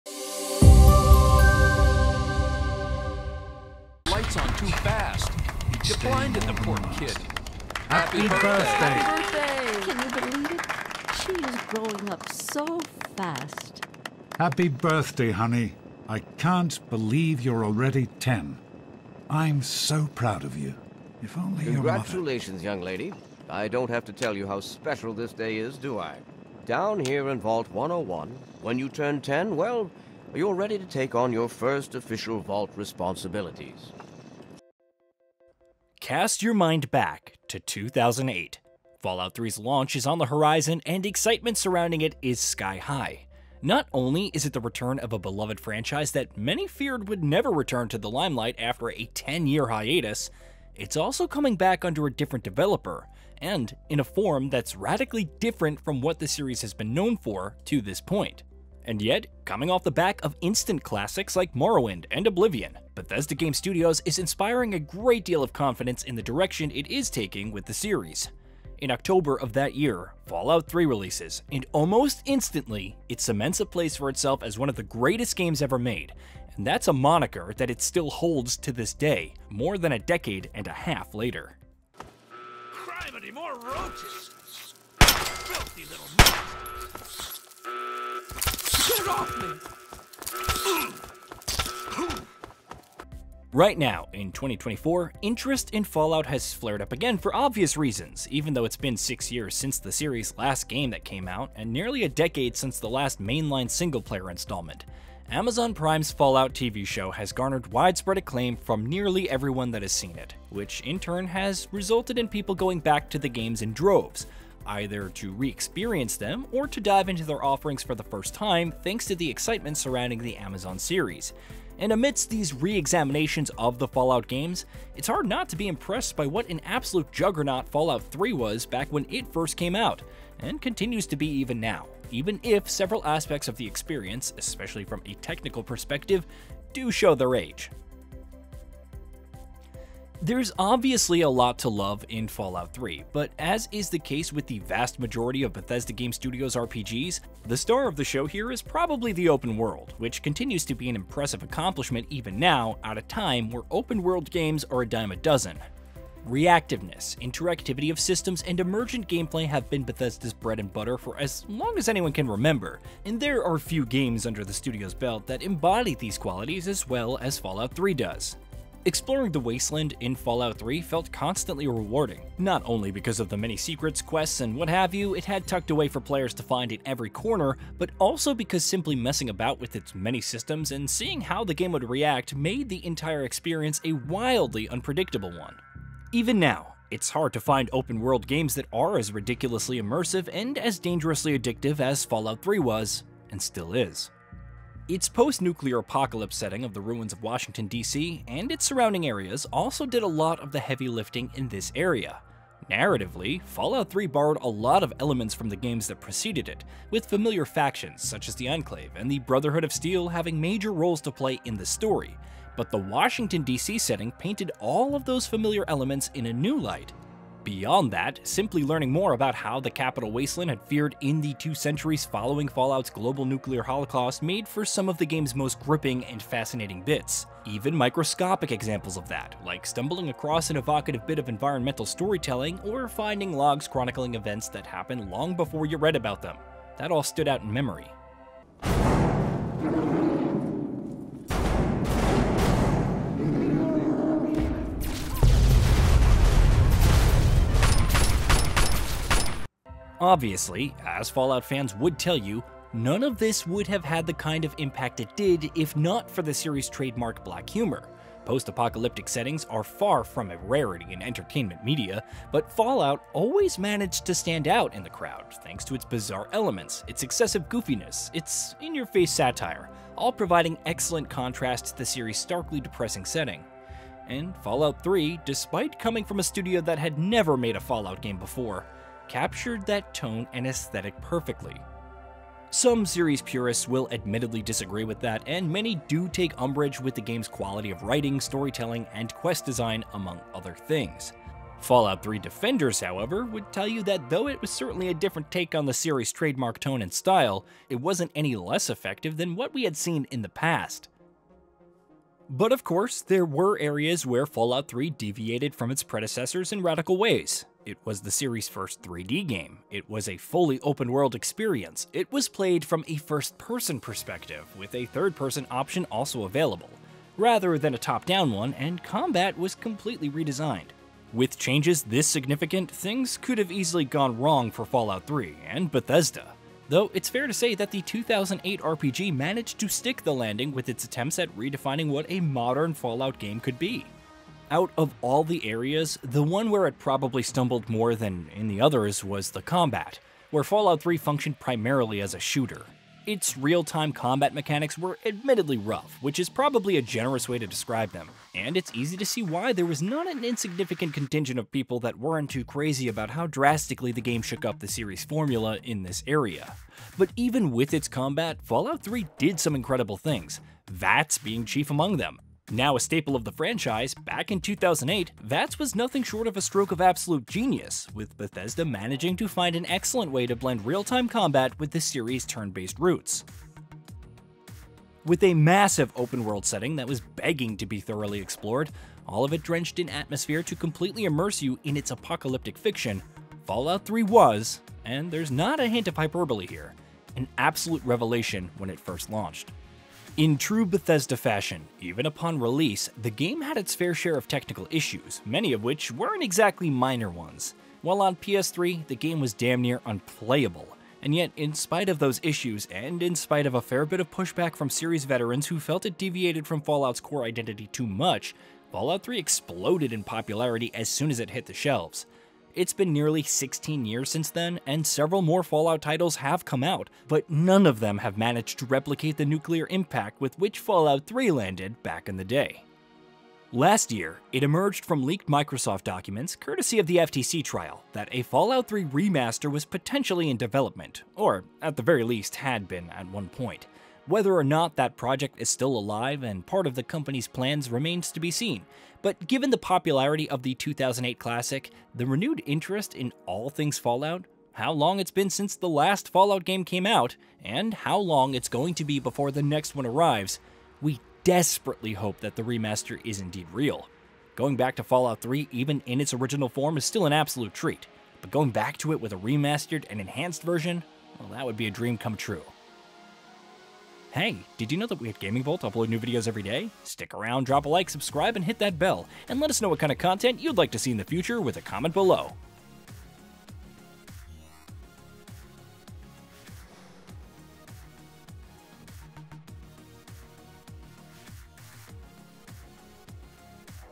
How Fallout 3 Took Gaming by Storm 16 Years Ago
From Morrowind to Oblivion to Skyrim and more, Bethesda Game Studios has delivered a number of standout, genre-defining open world RPGs over the years, but the one (or one of the ones) that the masses have collectively turned their attention to right now is 2008’s Fallout 3.

With Amazon’s Fallout TV series receiving widespread praise and reigniting general interest in the post-apocalyptic franchise, many are going back to Fallout 3 (and other titles in the series) either to experience it for the first time ever or to get another taste of its offerings.

And with that has come a reexamination of the landmark game and its many, many qualities. Here, we’re taking a retrospective look at the spectacular Fallout 3 and what made it the genre-defining game that we all now know it as.